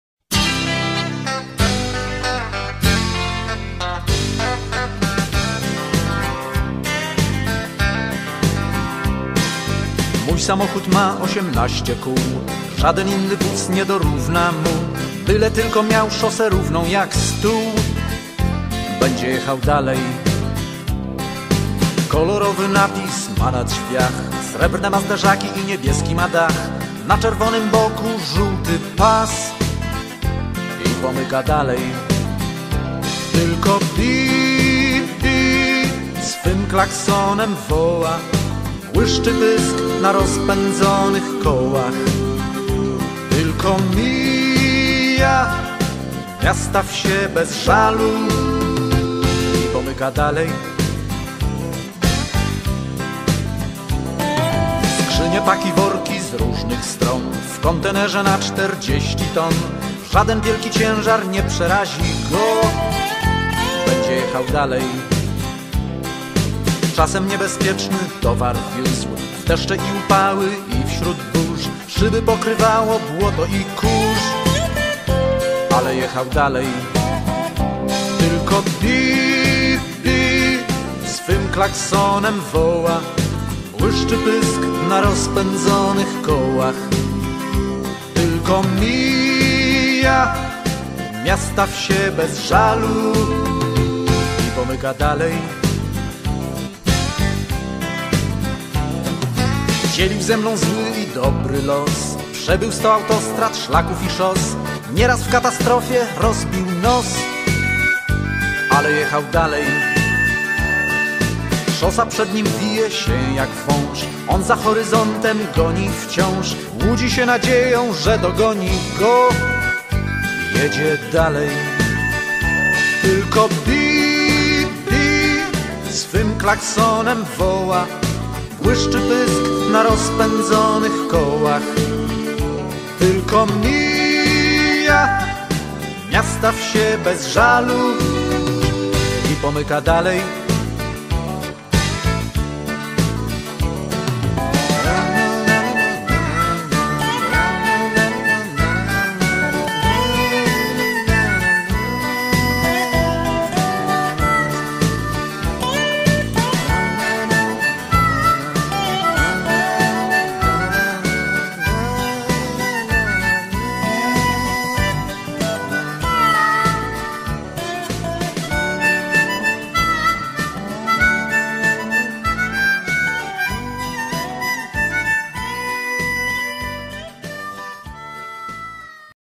Ciężarówki w trasie
Ciężarówki na drogach w Śląskim.Głównie droga 44, autostrada A4, droga 925 i droga S86